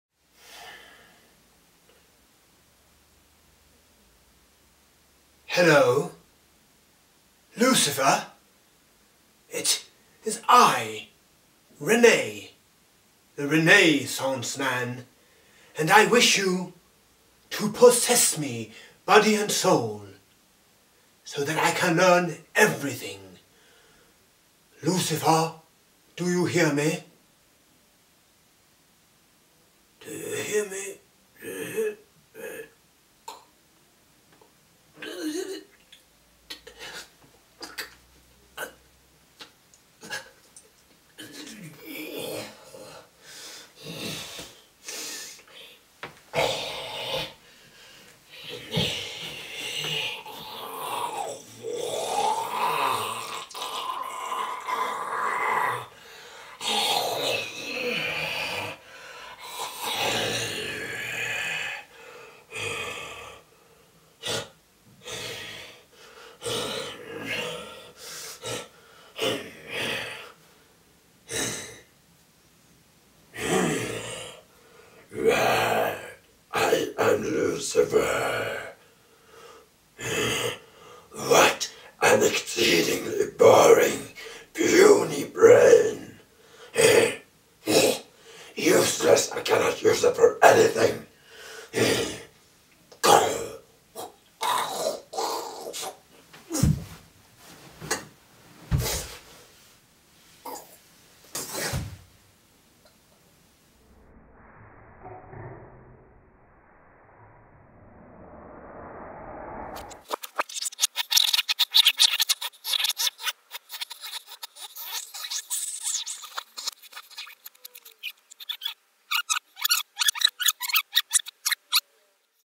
Renaissance Man and The Devil (Day 64 - Jens)
A character: René the renaissance man summons Lucifer and is a bit too successful.

 And like all stories, there had to come an end. In this particular instance, our hero did what many heroes do and most survive and are celebrated or even ordained for; he took himself all the way to the edge and beyond it. His hope was of course to return from the beyond, perhaps wearing a crown or other token of victory that he could wave at the world and have the world wave back at him for. But alas, not all heroes are successful and ours was one of the unlucky ones, so all we can do is hope that his deeds left something fruitful behind that can help others who come after to learn from his mistakes and go forward with more wisdom, creating less despair.